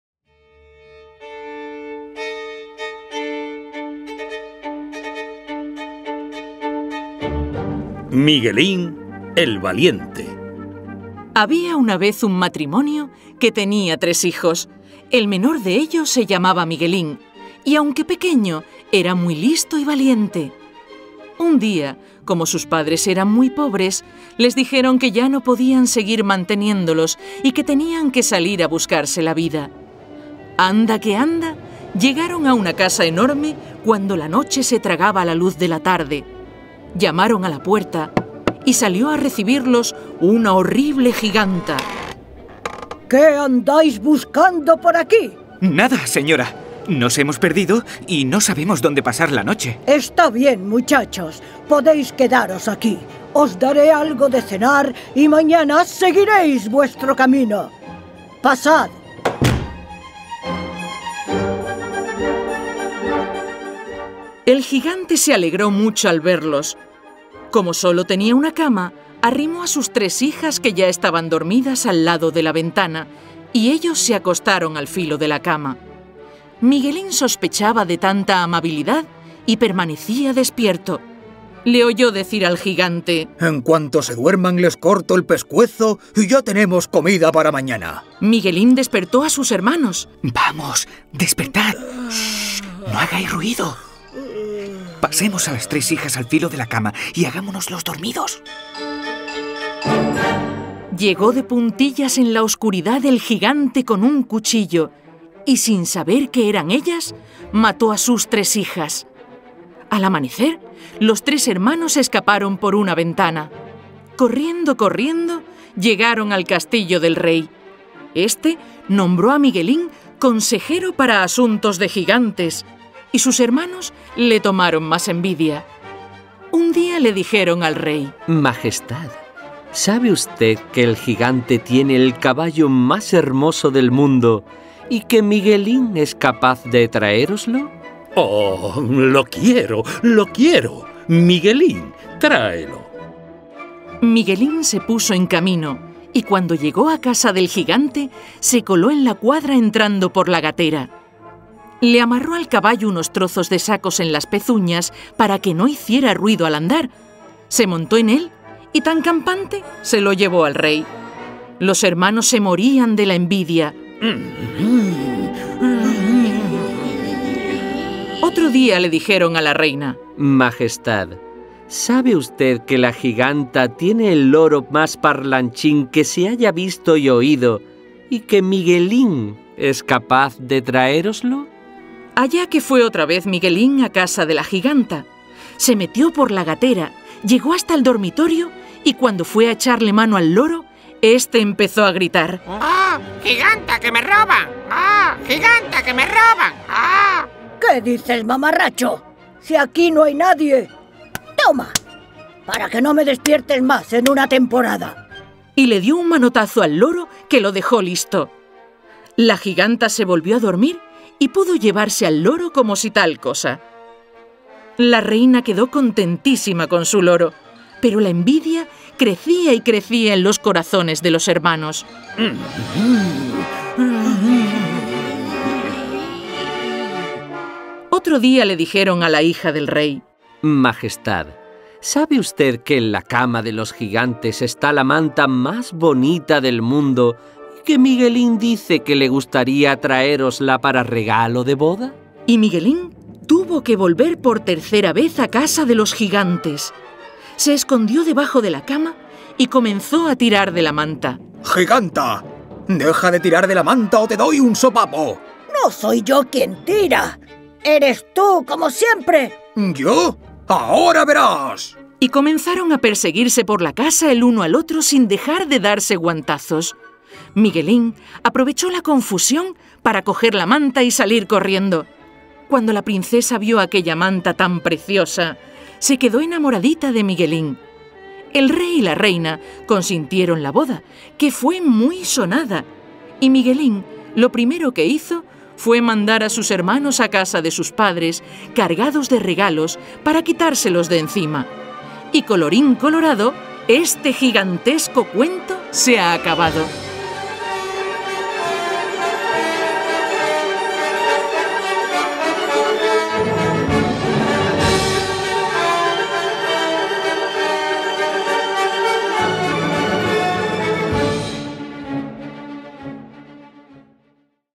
Miguelín el valiente
Miguelín es el más pequeño de tres hermanos. Sus padres no pueden seguir manteniéndolos y los mandan a ganarse la vida. Miguelín tiene que poner a prueba su inteligencia para sortear los peligros que le acechan.